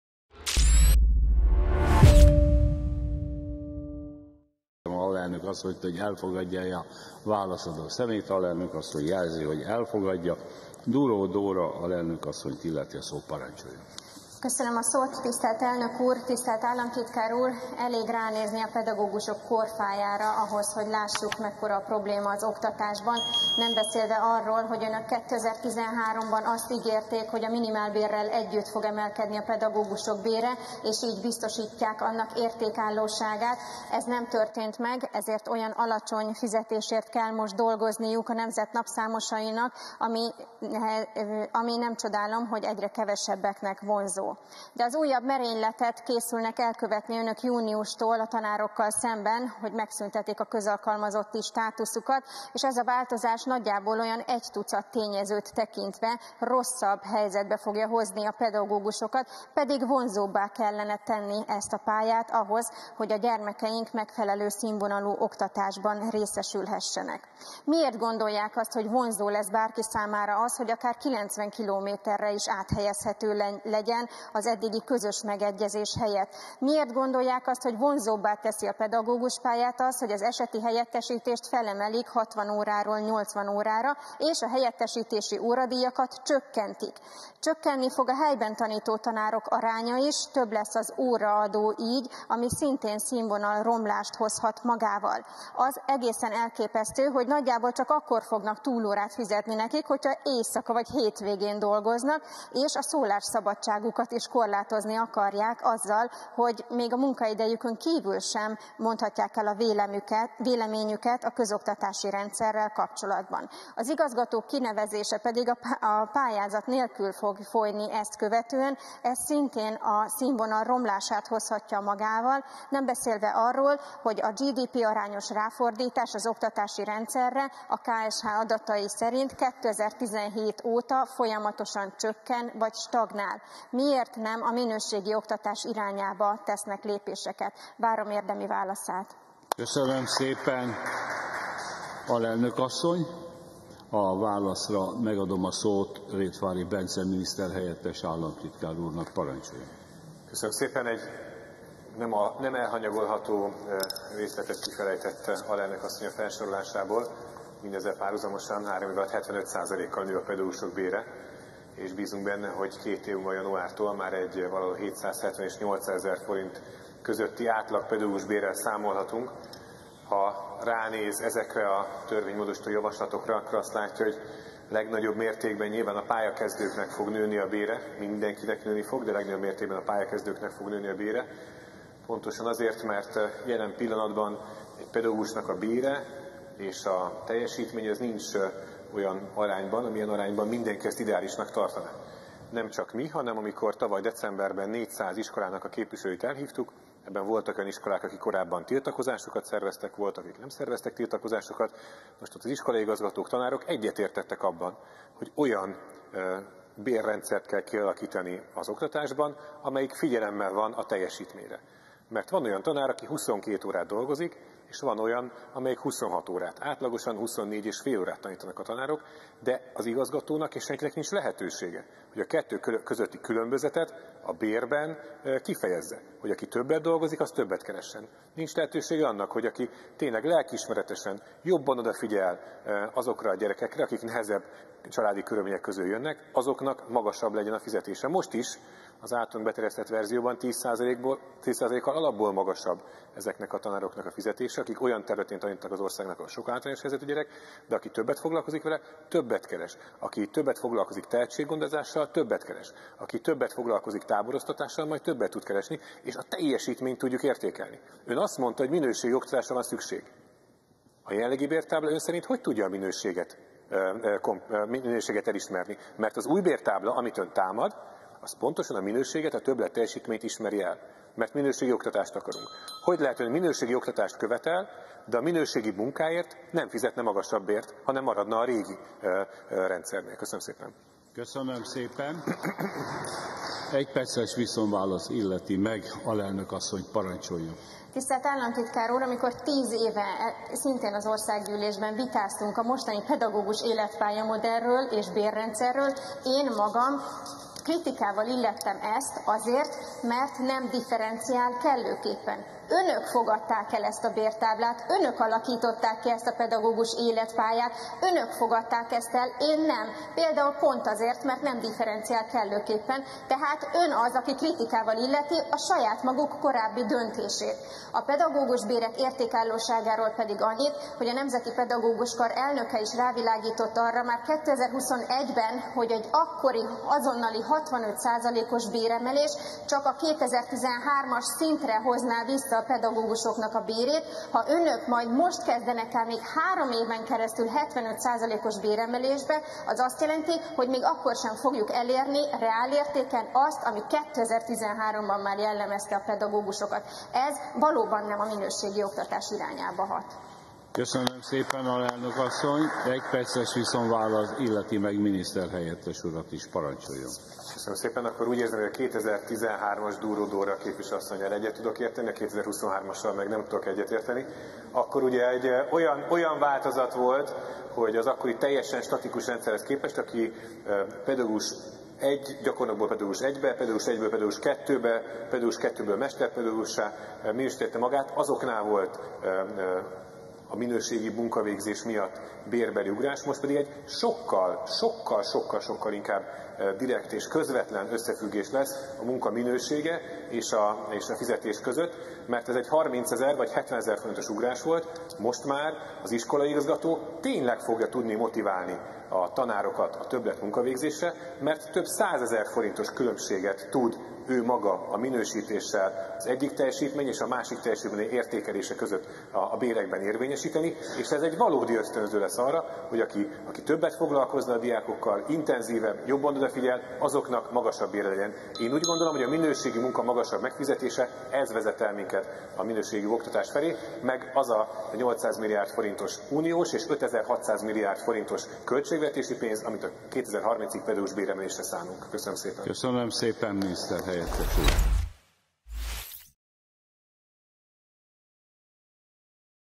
Mit művelnek még a pedagógusokkal?
Még rosszabb helyzetbe hozza a pedagógusokat a kormány, hiszen akár 90 km-re is áthelyezhetők lesznek, csökken a helyettesítési óradíj, ám ennek óraszámát megemelik. Magyarországon a tanárok 50%-a 50 év feletti, a fizetésük pedig bőven kevesebb mint felét éri a tíz évvel ezelőttinek. Ráadásul a GDP-arányos ráfordítás az oktatásra 2017. óta folyamatosan csökken.